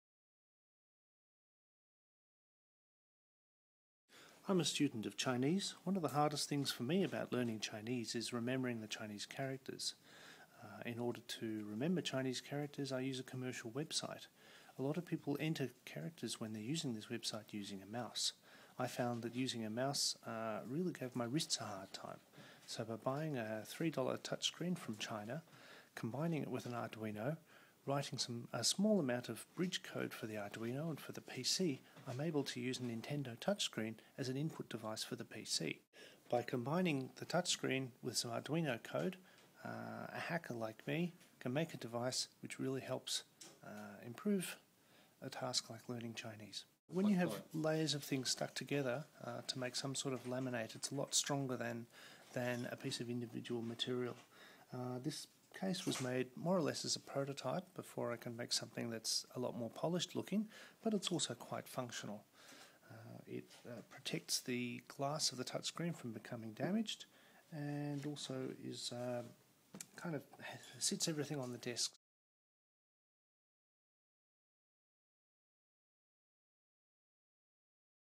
Chinese character input using Nintendo DS touchscreen and Arduino
Mitch Davis describes an Arduino-based device he designed and built, which allows him to enter Chinese characters on a Nintendo DS touchscreen.  This is a more comfortable and compact alternative to using a mouse.  The prototype case was hand-built using laminated cardboard.